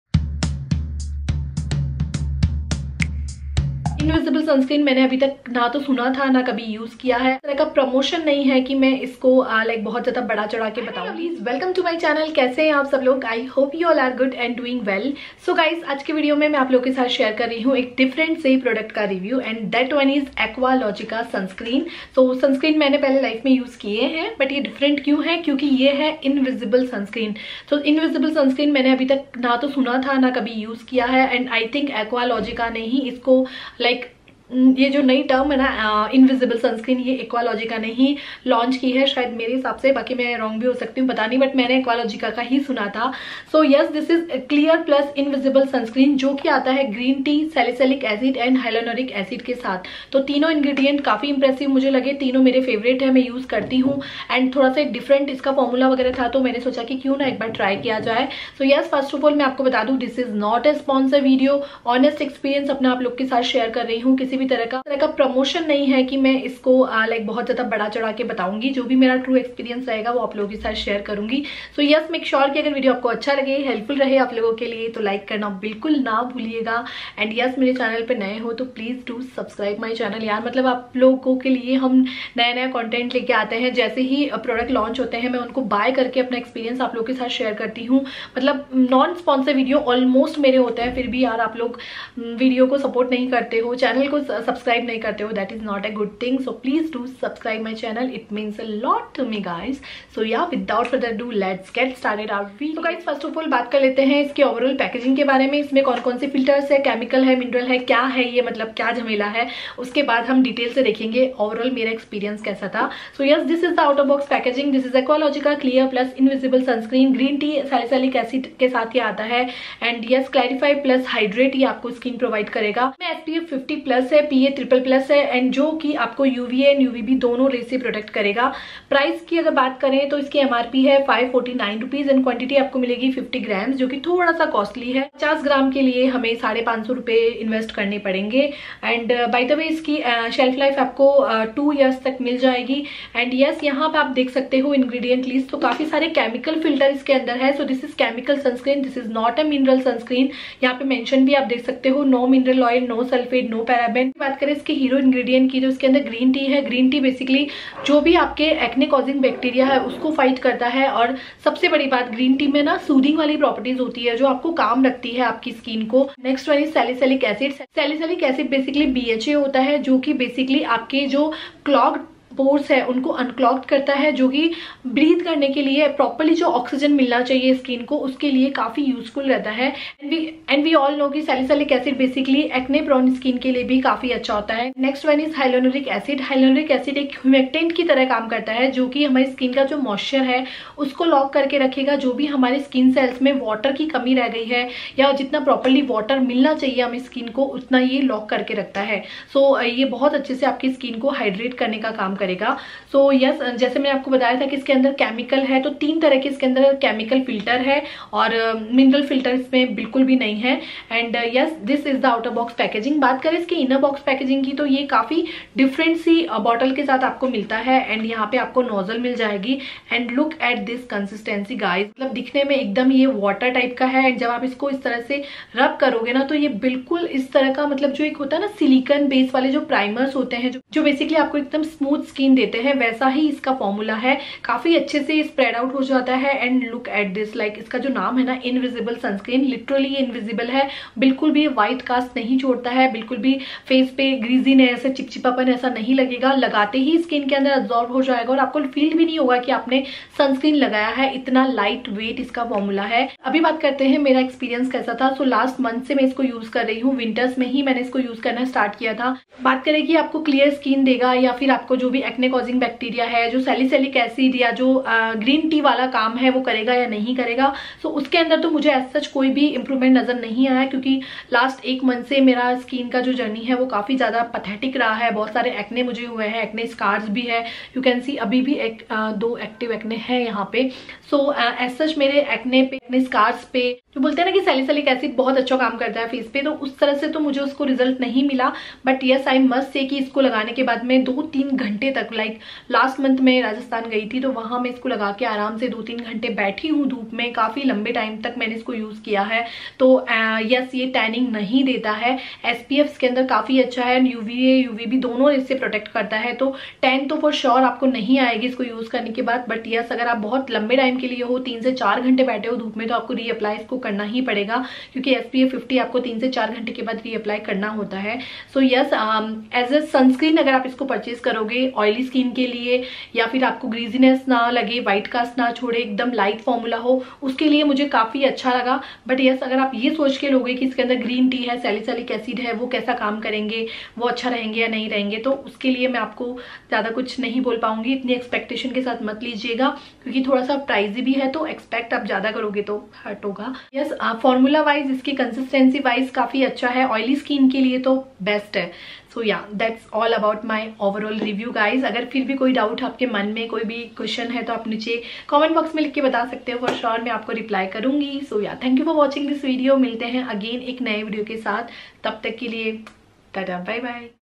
*New Aqualogica Invisible Sunscreen Review For Oily Acne Prone Skin || Aqaulogica Clear+ Sunscreen
Hello Everyone!
I used the aqualogica invisible sunscreen for more than 1 months. I am here to share my 1 months experience with you all with all live result in this video . I hope my review helps you all understand the product better. And you all know what to expect from this product , if you decide to get it.

xoxo.